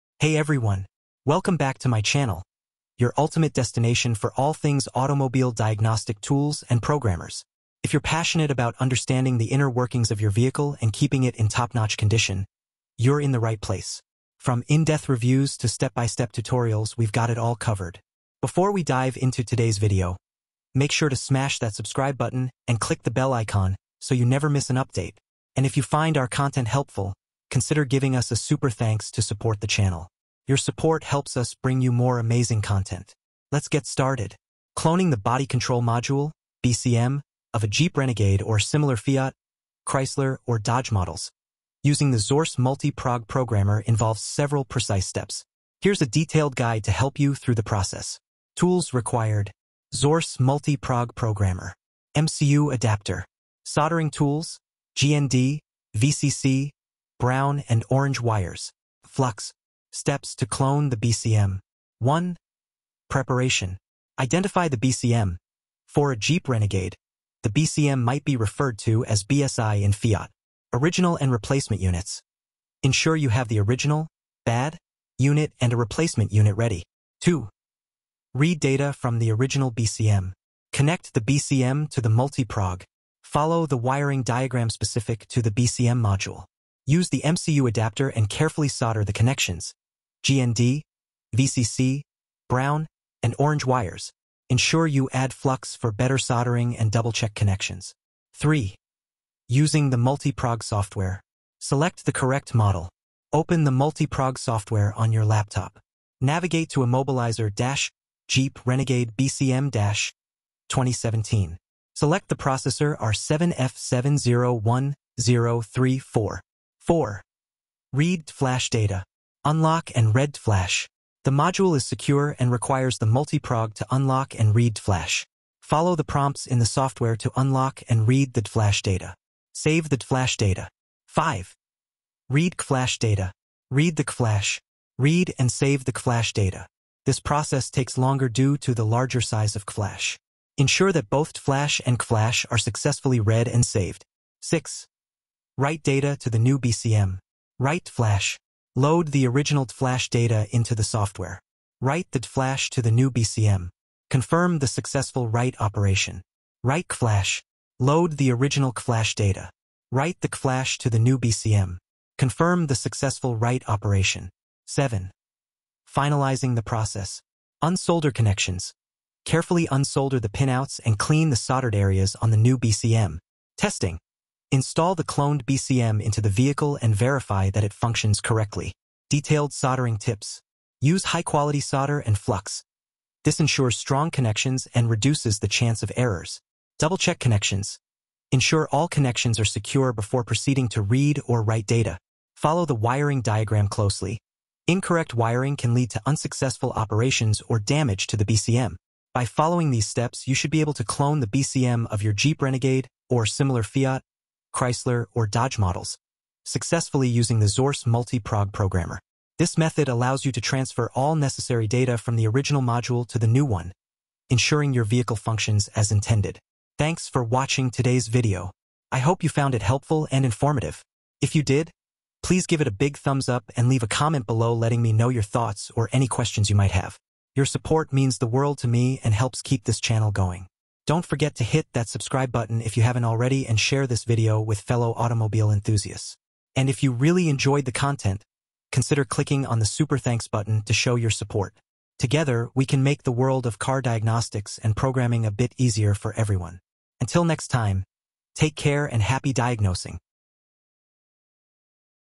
How to Clone Chrysler Jeep Dodge BCM with Xhorse Multi Prog
Learn how to clone the Body Control Module (BCM) of Chrysler, Jeep, and Dodge vehicles using the Xhorse Multi Prog programmer. In this tutorial, we focus on cloning the BCM of a Jeep Renegade, which is crucial for scenarios like flooded cars where the original unit needs replacement. The Xhorse Multi Prog offers secure reading and writing capabilities, ensuring a successful BCM cloning process. Follow along as we demonstrate the steps, from connecting the MCU adapter to reading Dflash and Cflash data, to successfully writing the cloned BCM data back to the module.

Key Features:
Clone Chrysler, Jeep, Dodge BCM modules with Xhorse Multi Prog.
Securely read and write Dflash and Cflash data for BCM cloning.
Detailed tutorial on wiring, software setup, and step-by-step cloning process.
Ensure reliable BCM replacement in vehicles like the Jeep Renegade with this comprehensive guide.